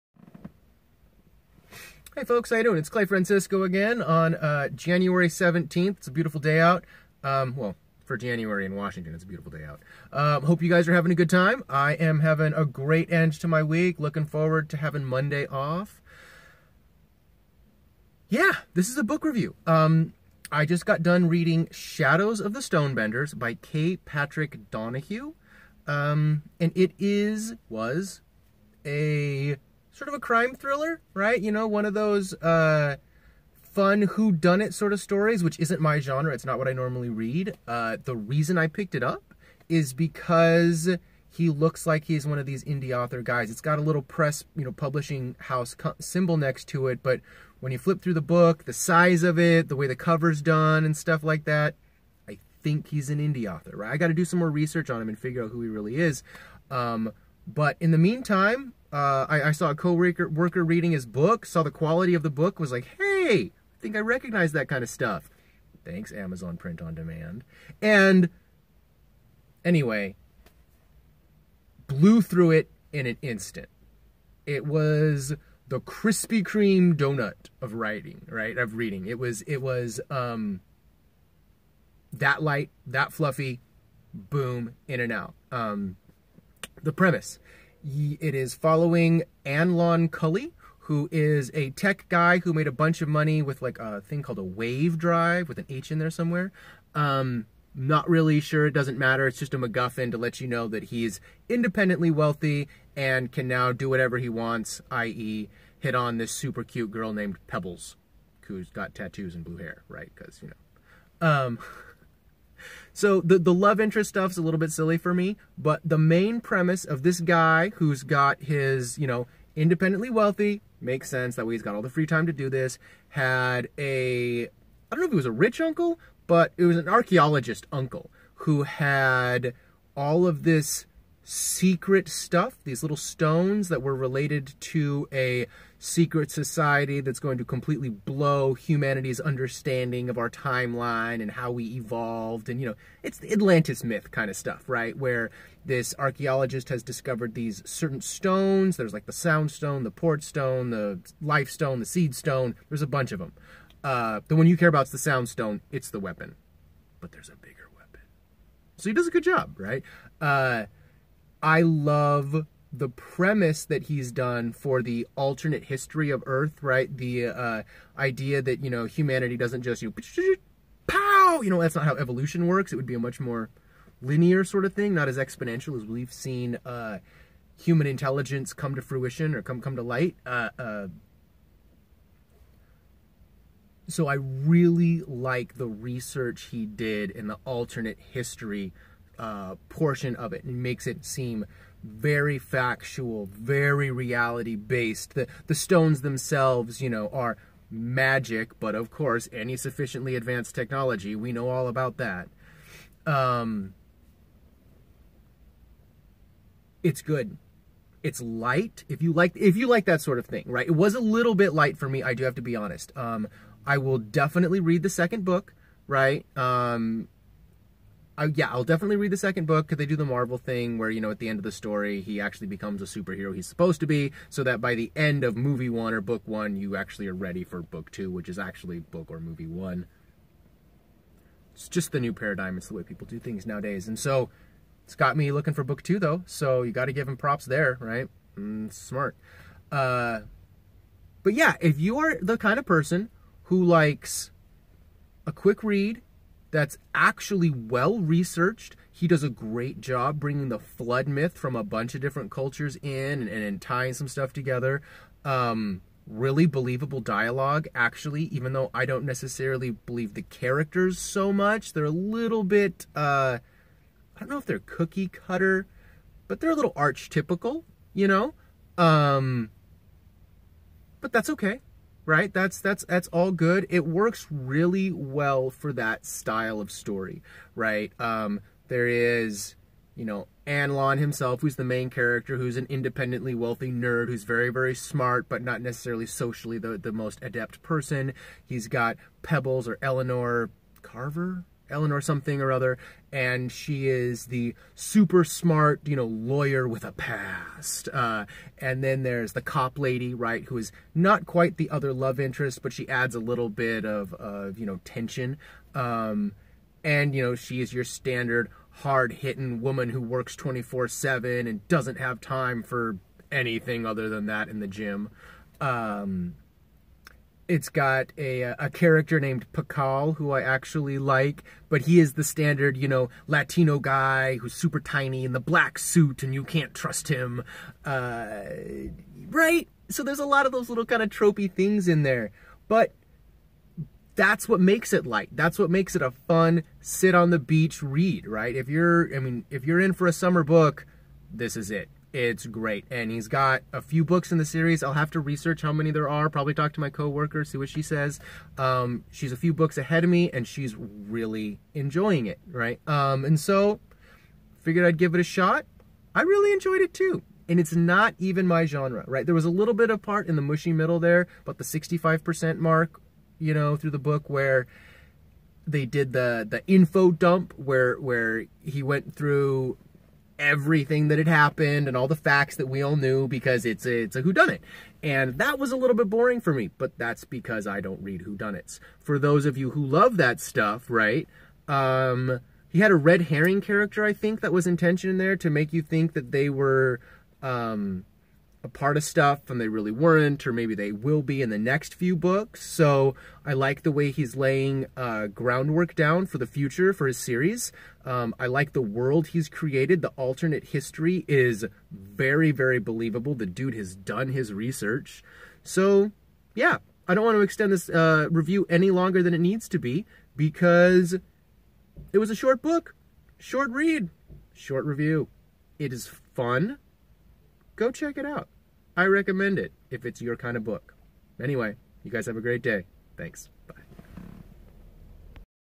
Shadows of the Stone Benders, by K. Patrick Donoghue - book review
Book review of the crime thriller: Shadows of the Stone Benders (The Anlon Cully Chronicles Book 1), by K. Patrick Donoghue.

This is a fun, light read, that was surprisingly well researched, and happens to be on what is quite possibly my favorite scientific topic: Neolithic man's technological abilities -- focusing on their understanding of and relationship to the natural world. I love his take on it and the exploration he puts forth of modern man's exploration of what is basically "alien" technology from our ancient ancestors. Fun stuff!

Curious About What I'm Doing?

Find my Book on Amazon for only $0.99 - A Tale of Karg: The Viper and The King

-Management.